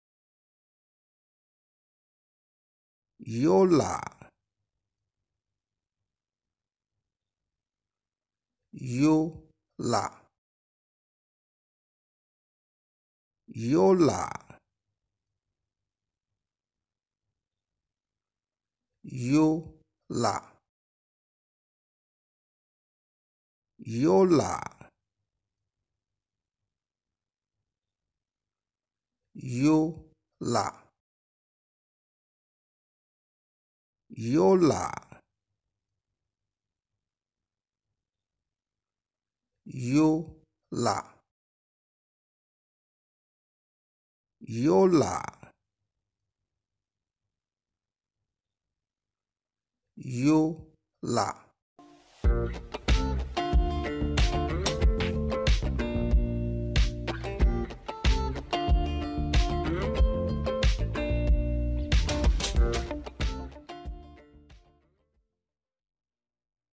How to pronounce Yola | Location, Culture, Tribe, Language, History, NIGERIA
Yola, meaning 'Great Plain' or 'Vast Plain Land', is the largest city, capital city and administrative center of Adamawa State, Nigeria. Located on the Benue River, it has a population of 336,648. Yola is split into two parts. Wikipedia
Area: 831 km²
Elevation: 599 m
Hotels: 3-star averaging ₦5,000. View hotels
Weather: 29°C, Wind NE at 11 km/h, 15% Humidity weather.com
Climate: Aw